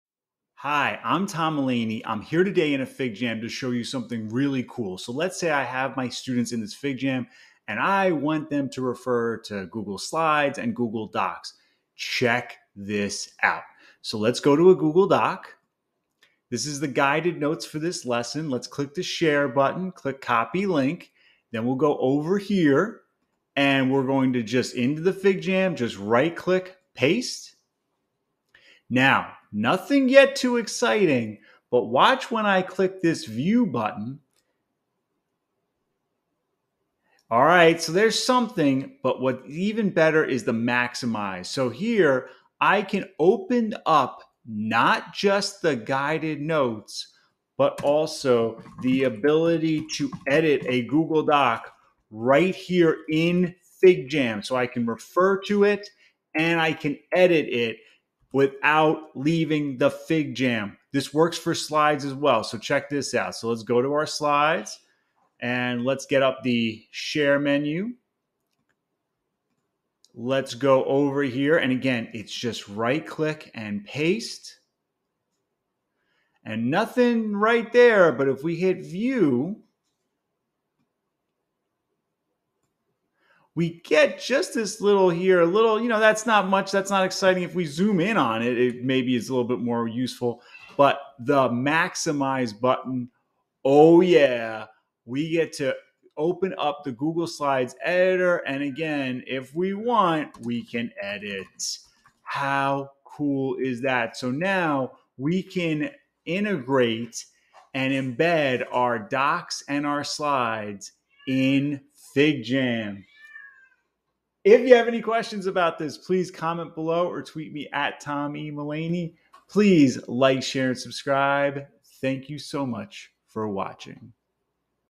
FigJam - View and Edit Google Docs and Slides
Copy and paste a Google Doc or Google Slides link into a FigJam. You can view and edit the files!